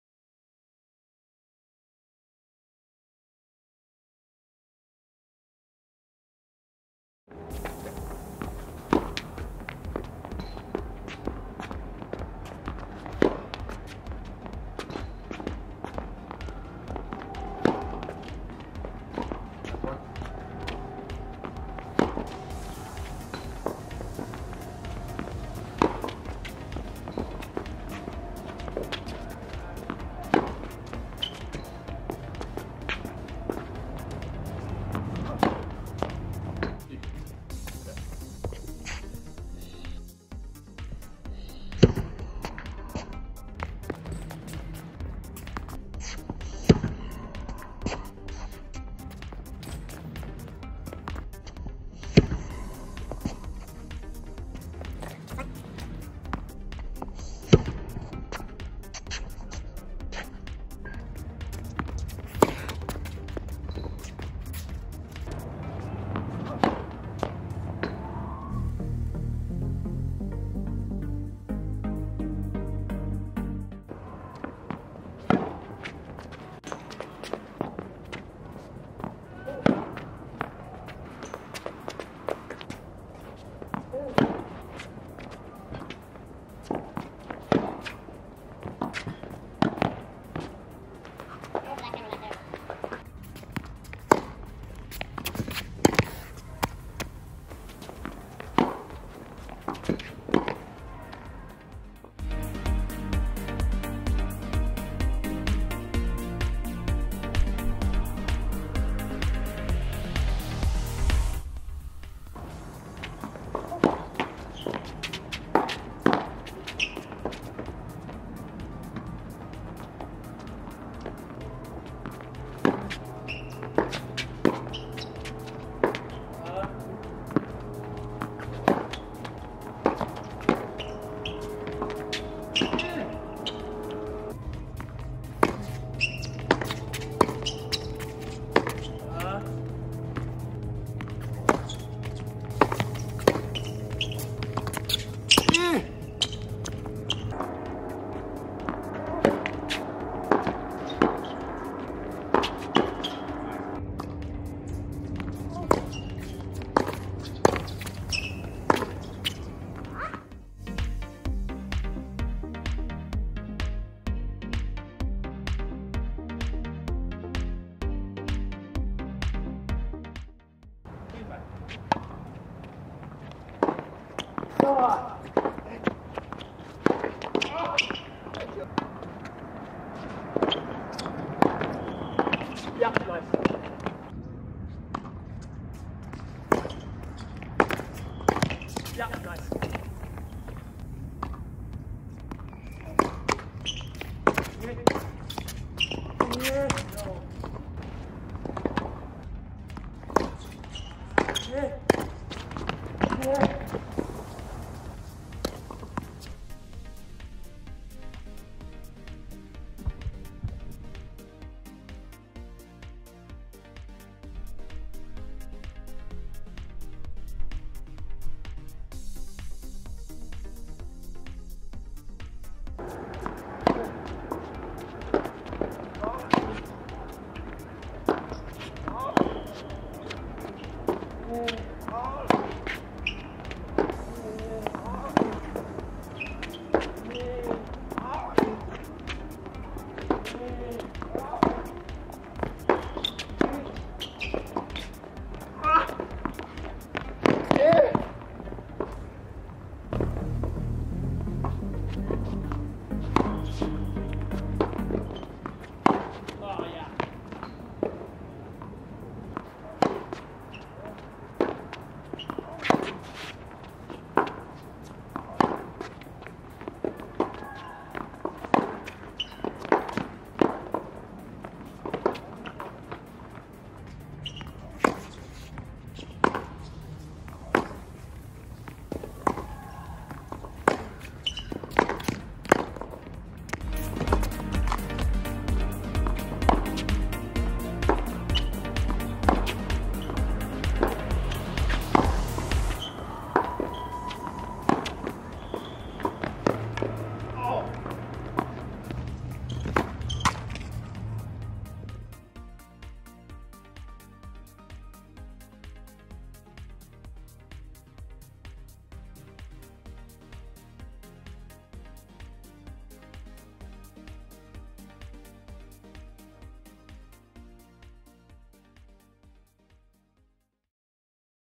Tennis Approach Shots - Hit Better Approaches With 5 Simple Drills
Approach shots can be tricky for every player. Here are my favorite drills to sharpening up my approaches. I don’t focus on technique; I focus on creating the right swing thought through the shot. Hope you enjoy it. Let us know what would you like to see from us!